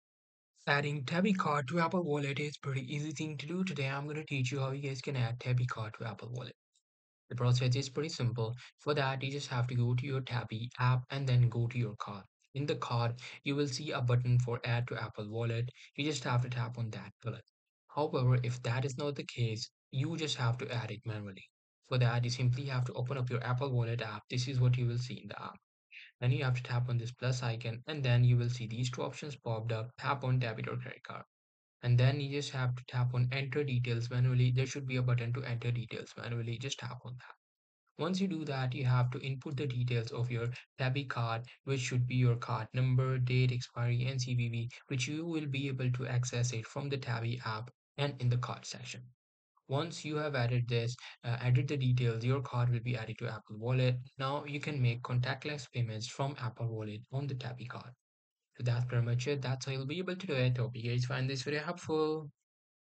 How To Add Tabby Card In Apple Wallet (2024) - FULL GUIDE!
In this video I will show you how to add tabby card in apple wallet